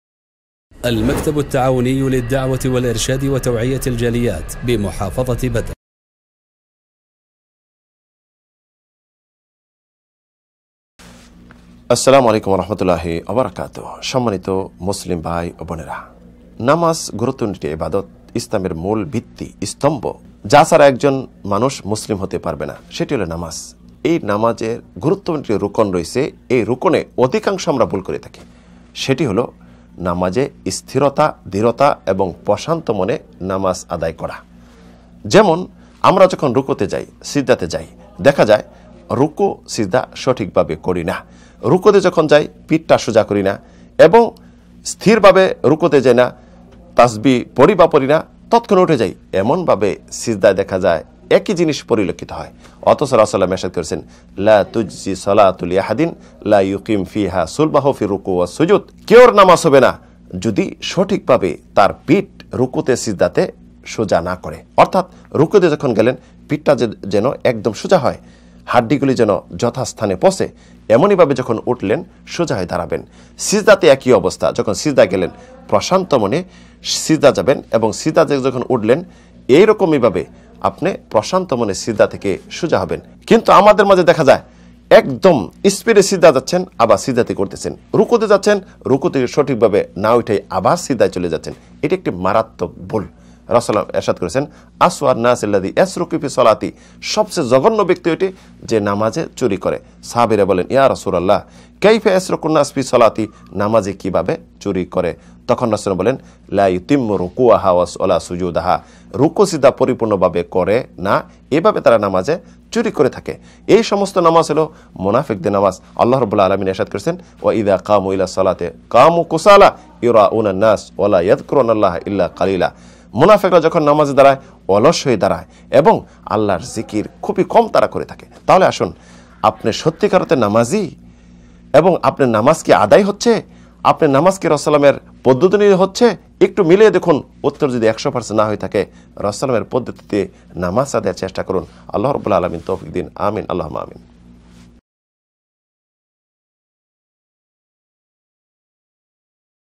বিশ্ব নবী কাকে জঘন্যতম চুর বলে আখ্যা দিয়েছেন? BANGLA WAZ LECTURE by Rafiq Bin Lokman Madani
বিশ্ব নবী কাকে জঘন্যতম চুর বলে আখ্যা দিয়েছেন?
Who is the world's worst thief?
বিশ্ব নবী কাকে জঘন্যতম চুর বলে আখ্যা দিয়েছেন জানেন কি ?
জঘন্যতম চুরি কি ?
সকল মুসলিমের অবশ্যই জানা জরুরী !